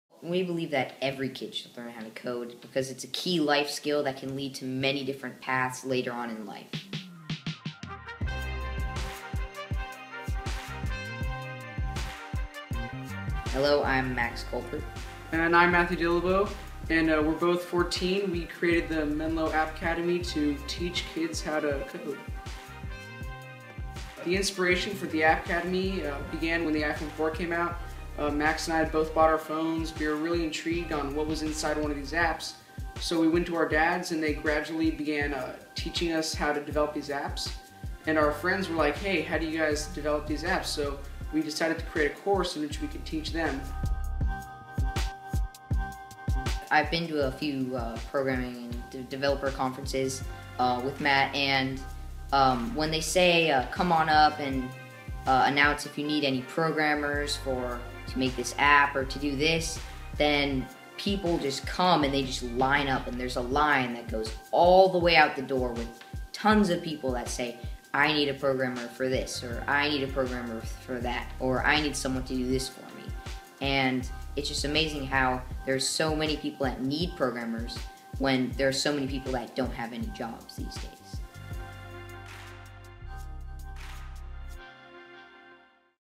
Menlo App Academy: Every kid should learn to code
Max Colbert and Matt Dillabough, two teenage founders of Menlo App Academy, believe that doing is like learning how to read, everyone kid should do it.